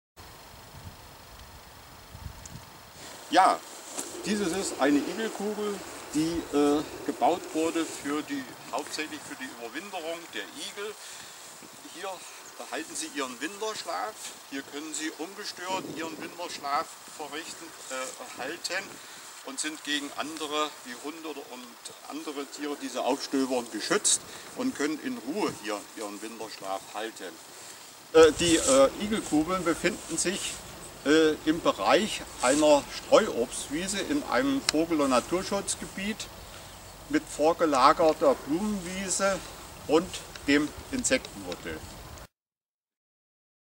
Igelkugel in Eschwege
Diese Igelkugel bietet einen sicheren Unterschlupf für Igel in der Zeit des Winterschlafs. Die Kugel ist aus Altholz, Reisig und Laub von Vogelschutzbeauftragten der Stadt Eschwege gebaut worden.